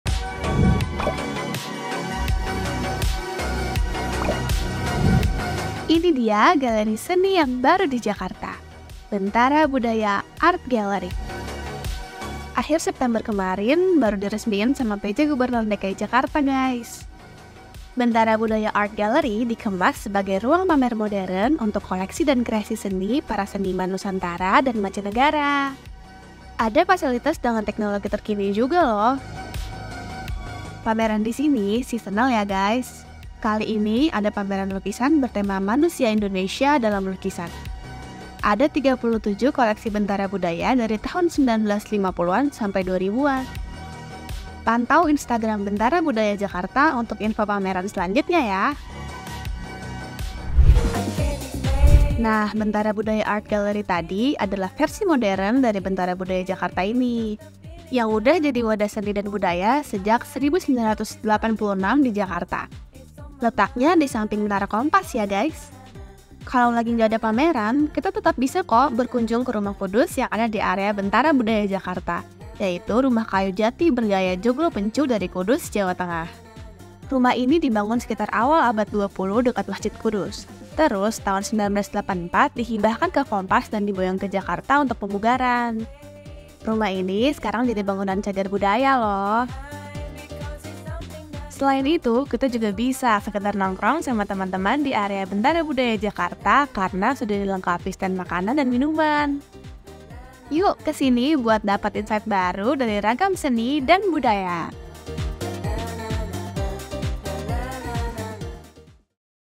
Explore Jakarta edisi Bentara Budaya Jakarta
Bentara Budaya Art Gallery.

Galeri seni yang baru diresmikan oleh Pj. Gubernur DKI Jakarta, pada 26 September 2023, berlokasi di belakang Menara Kompas.

Bentara Budaya Art Gallery dikemas sebagai ruang pamer modern untuk koleksi dan kreasi seni para seniman Nusantara dan mancanegara. Ada fasilitas dengan teknologi terkini juga lho.

Pameran di sini seasonal, kalau lagi tidak ada pameran, kamu bisa berkunjung ke Rumah Kudus yang ada di dalam Bentara Budaya Jakarta. Atau sekadar nongkrong sama teman-teman di sini juga bisa.

Yuk, ke sini buat dapat insight baru dari ragam seni dan budaya!

#19/10/2023

Pemilik Video : Diskominfotik Pemprov DKI Jakarta (Bidang Komunikasi Publik dan Informasi Publik) - Seksi Layanan Hubungan Media

cePSA